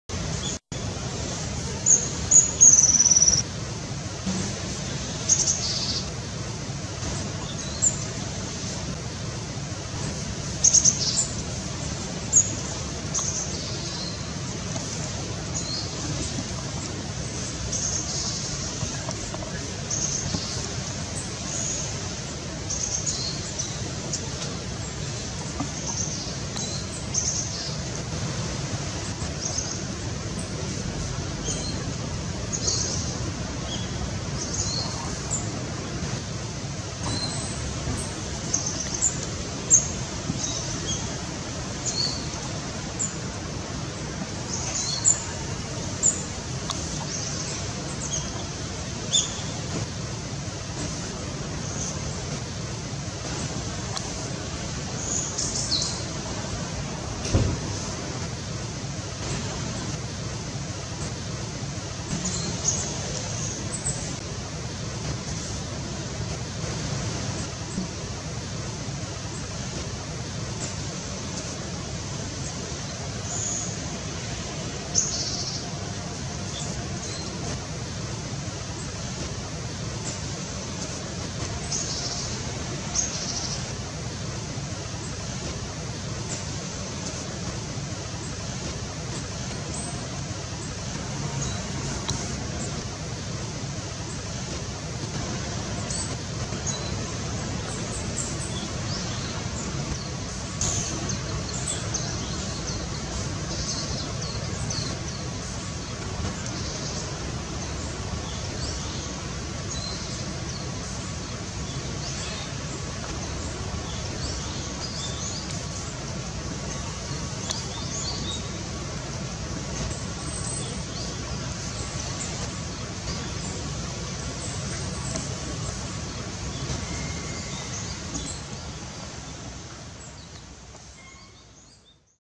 東京生き物図鑑 神田川のコゲラ
2016年10月中旬に杉並区内の神田川沿いの公園で目撃したコゲラ（Dendrocopos kizuki）です。